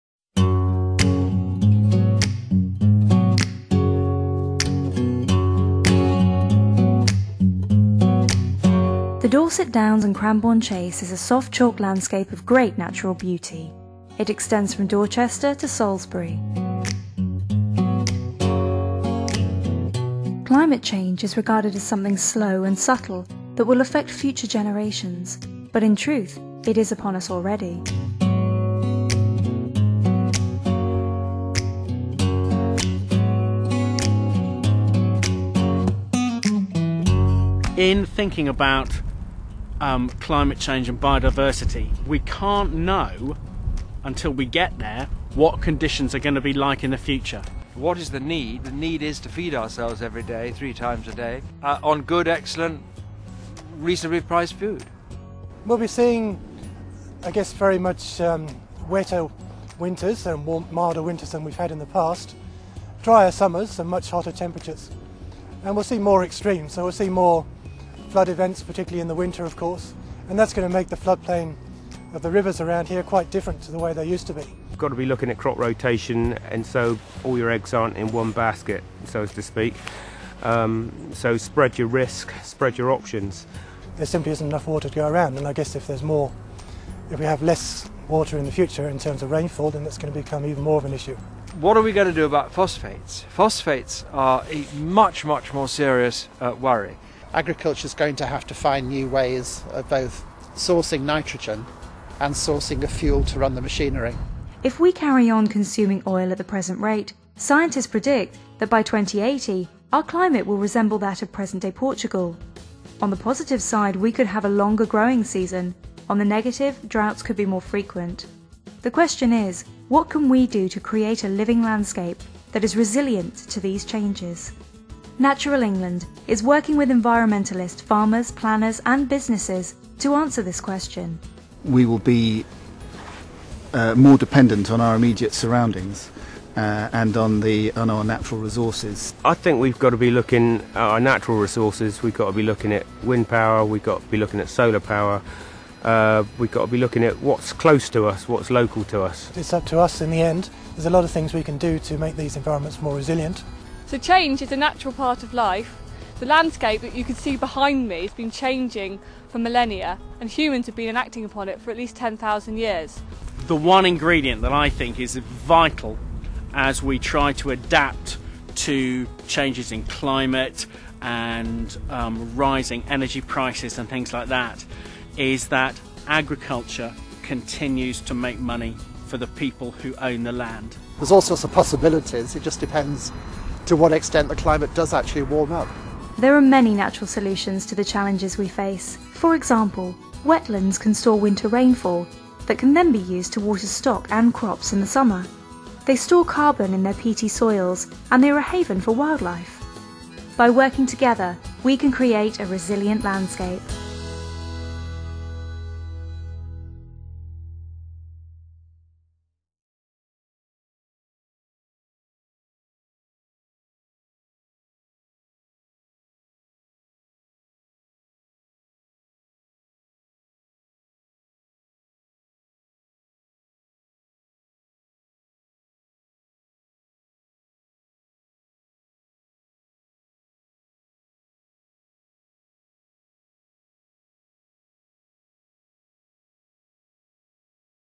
Natural solutions to climate change
By the year 2080, the climate of the Dorset Downs and Cranborne Chase might resemble that of present day Portugal. Natural England brought people together to understand the implications for the natural environment and highlight the practical land use/management solutions that will create a high quality, resilient landscape for nature and people.